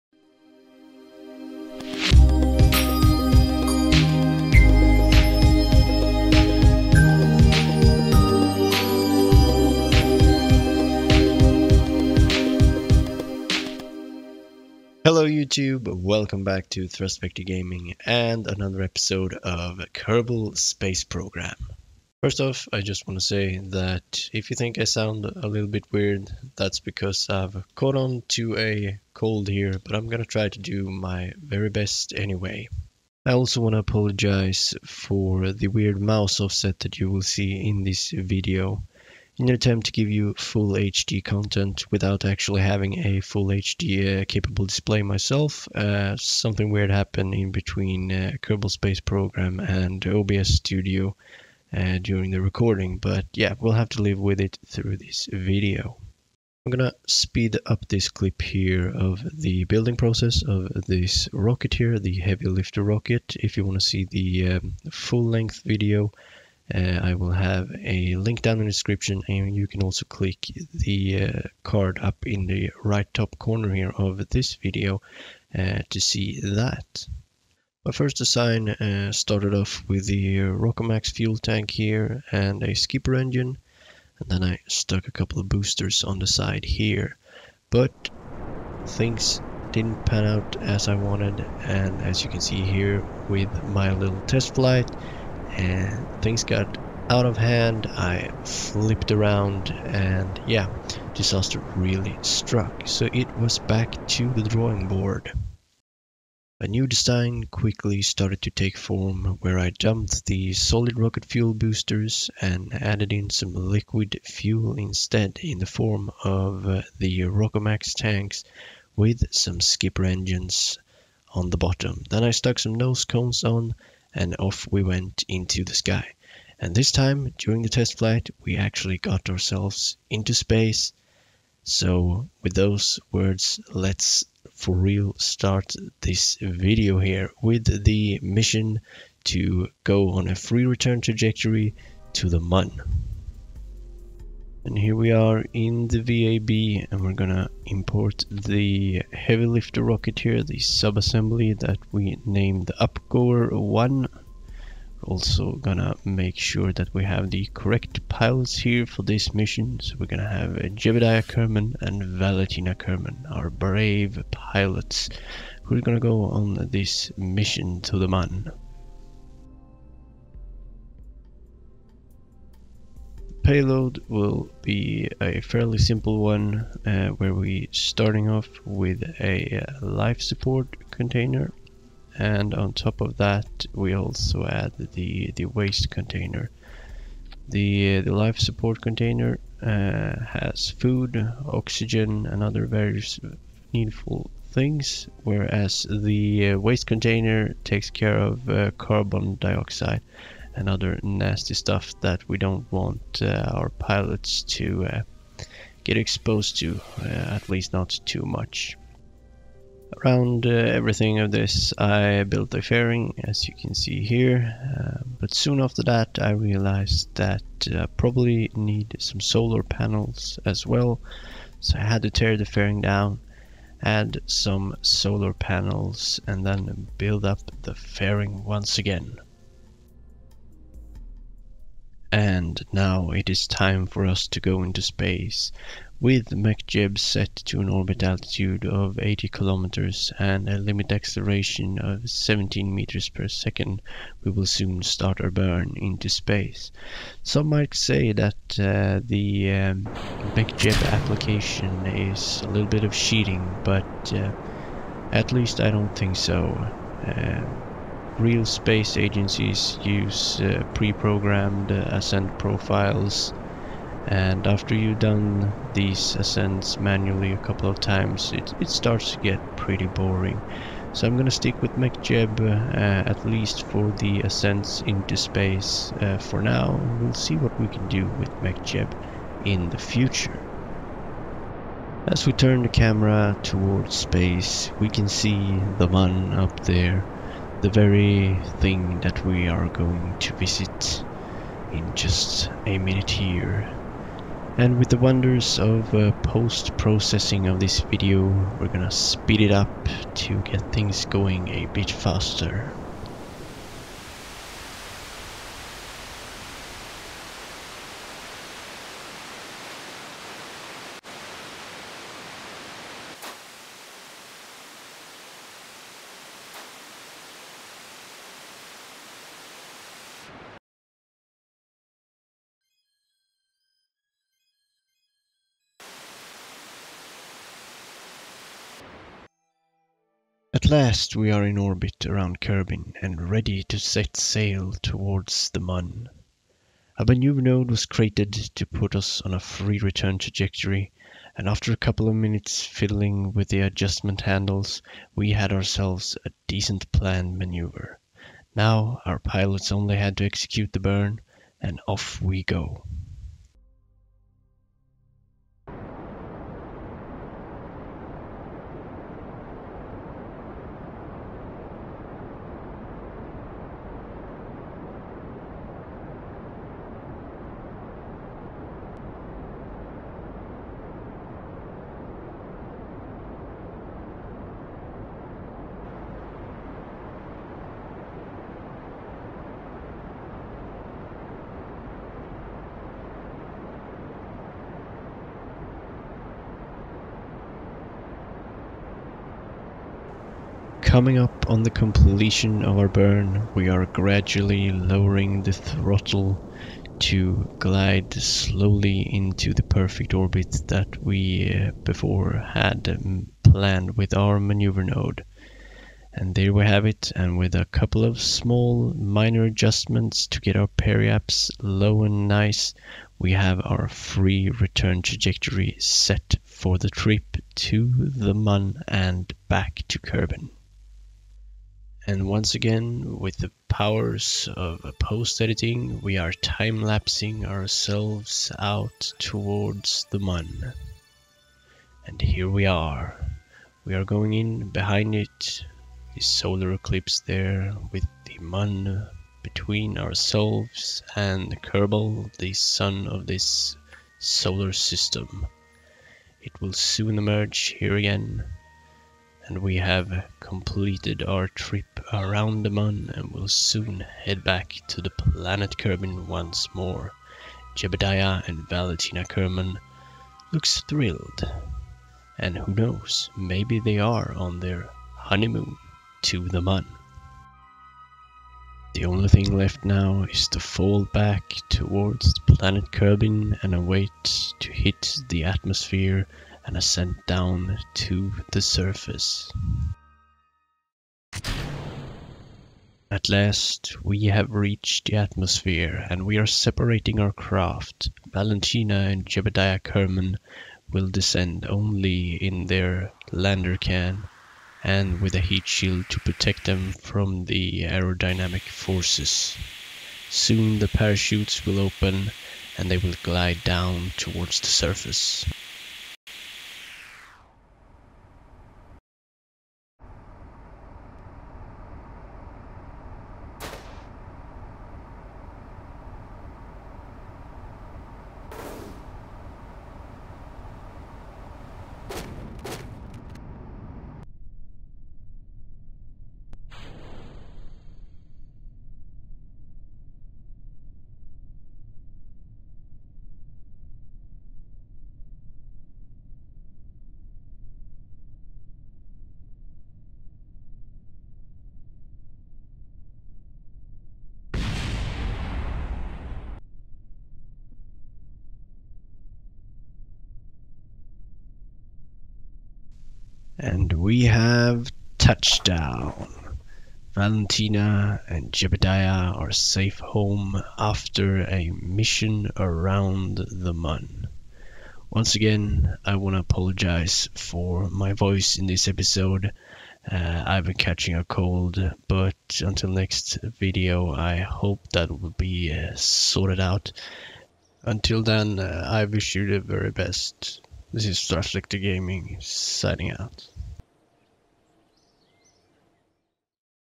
TVG Space Agency E02 - The Honeymoon To The Mun
In todays episode Jeb and Val takes a trip to the Mun and back using a free return trajectory.